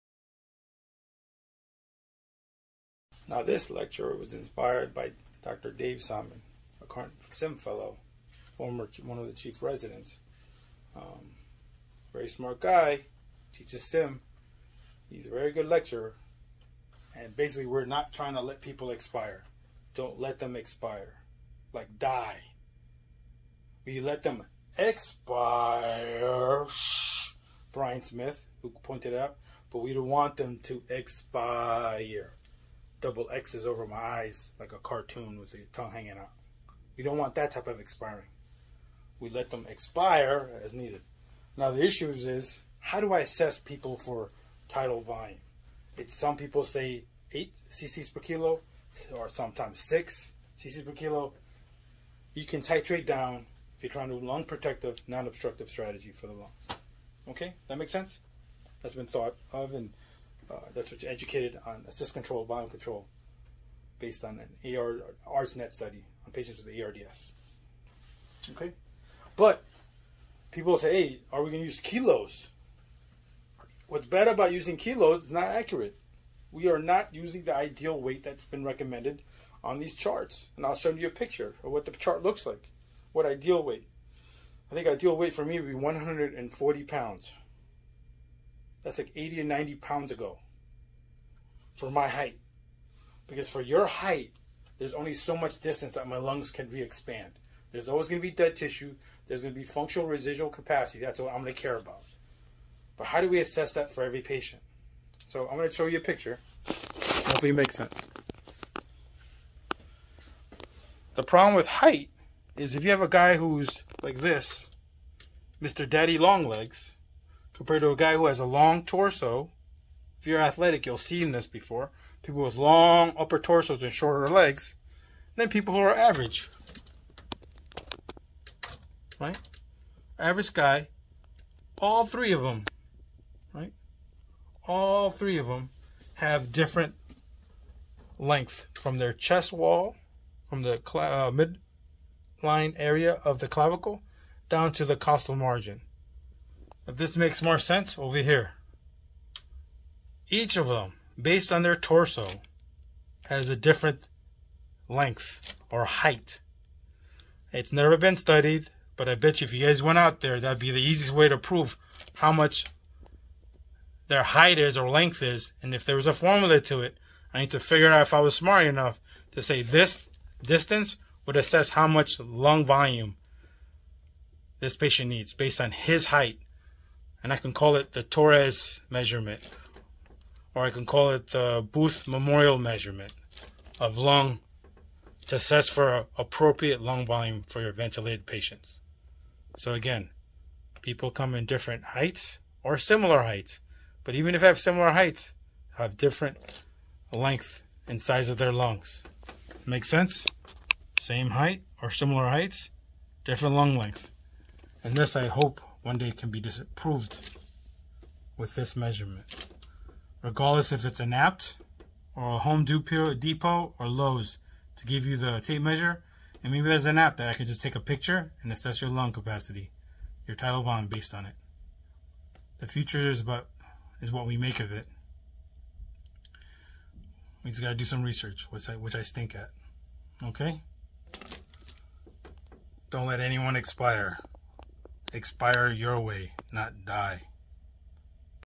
Ideal Tidal Volume to be decided on the Ventilator
Novel way to think of it.
I need to prove it with my pulmonary colleagues.
Correlate pulmonary function tests to ideal tidal volume and figure how the length from midclavicular locatiom from clavicle to anterior rib/costal margin?